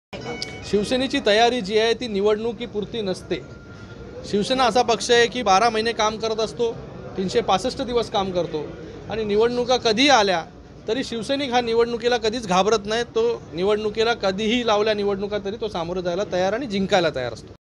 KALYAN | निवडणूक कधीही लागली तरी शिवसैनिक सामोरे जायला आणि जिंकायला तयार - खासदार श्रीकांत शिंदे
आज सुप्रीम कोर्टातील ओबीसी आरक्षनाच्या याचिकेवर झालेल्या निर्णया समयी कोर्टाने दोन आठवड्यात राज्यातील राखडलेल्या निवडणुकांचा कार्यक्रम जाहीर करावा असे आदेश निवडणूक आयोगाला दिले आहेत.यामुळे निवडणूकाचा तारखा कधीही जाहीर होऊ शकतात यामुळे निवडणूका लागल्या तर शिवसेनची तयारी असेल का.यासंदर्भात प्रश्न खासदार  शिंदे यांना विचारला असता. शिवसेनेची तयारी ही निवडणुका पुरती नसते तर शिवसेना हा पक्ष १२ महिने, ३६५ दिवस काम करत असतो.तरी निवडणूक कधीही आल्या तरी शिवसैनिक निवडणुकीला कधीच घाबरत नसतो. कधीही निवडणुका लावल्या तरी त्याला सामोरे जायला आणि जिंकायला तयार असतो असे खा शिंदे यांनी पत्रकार परिषेद वेळी सांगितले. तसेच लवकरच कल्याण आणि अंबरनाथ तालुक्यातील पाण्याचा प्रश्न सुटेल असे सांगितले.

BYTE  :  खासदार  श्रीकांत शिंदे